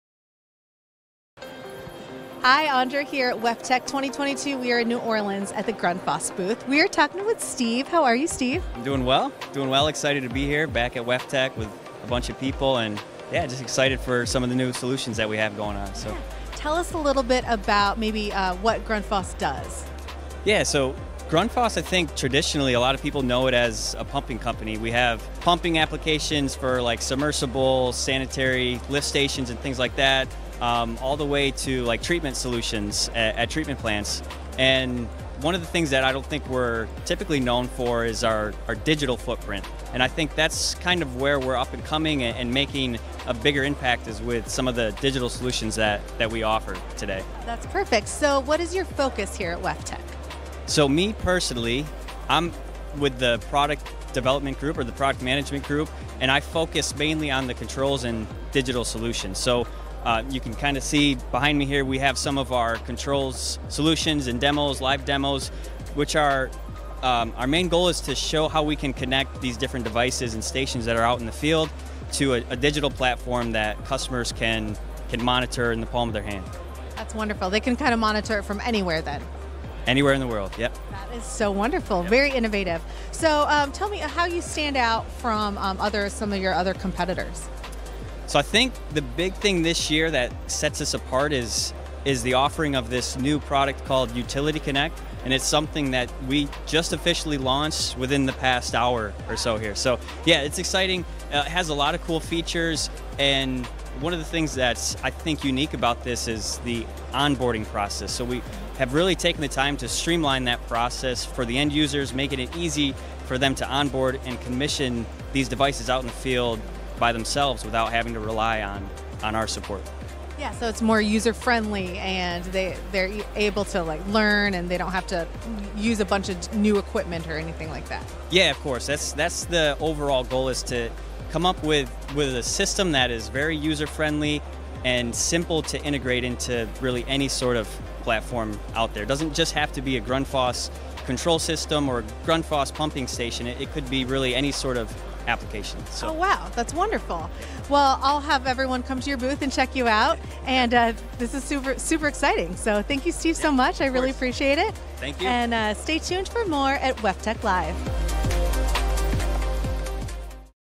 Around the Exhibition: Grundfos
WEFTEC Live visited Grundfos in Booth No. 2209 and 3507 to hear about the launch of Grundfos Utility Connect, which the company launched on Monday at WEFTEC. Hear more about their digital solutions and how their main goal is to show how to connect devices and stations in the field to a digital platform that customers can monitor and hold in their palms. The company seeks to make onboarding and commission as easy as possible.